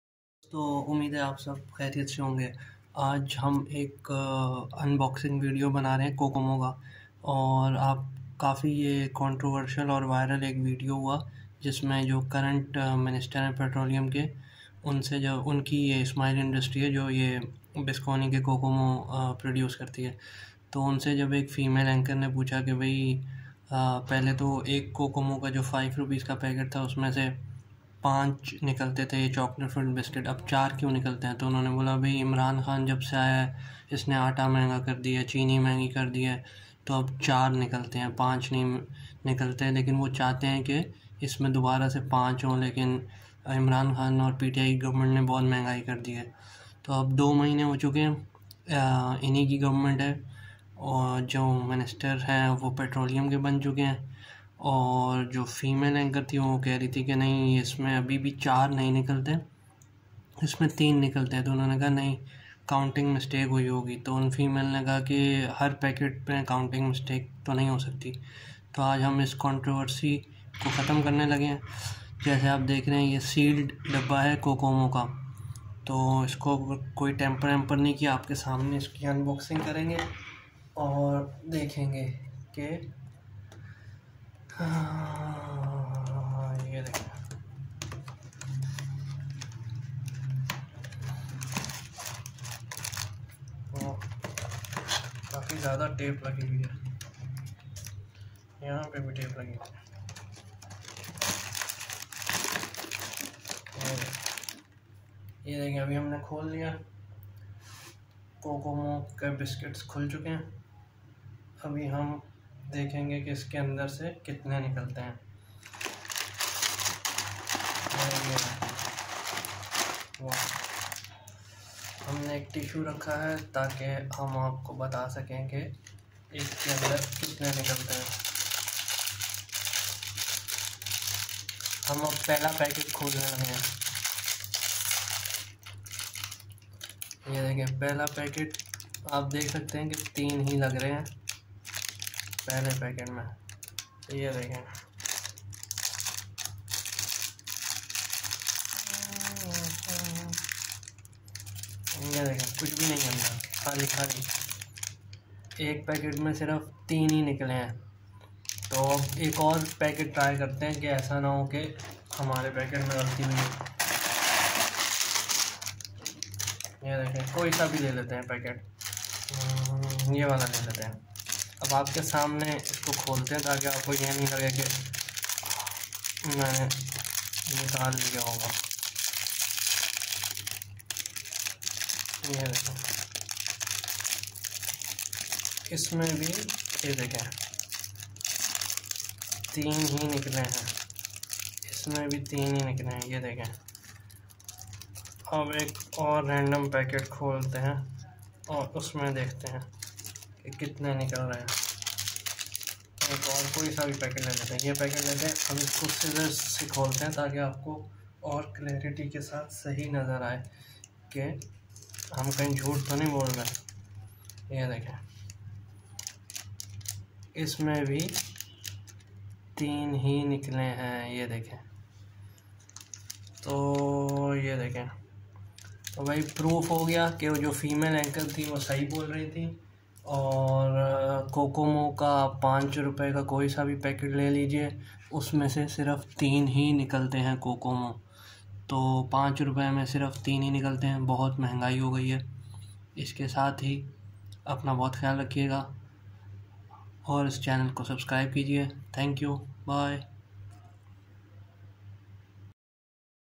Bisconni Cocomo double chocolate
Bisconni Cocomo double chocolate most authentic review following the controversy that how many Cocomo are in Rs 5 packet as per Mr. Miftah Ismail owner of Ismail Industries who produce Bisconni Cocomo there are 4 but as per anchor who interview him there are 3.

See the full video to know the truth.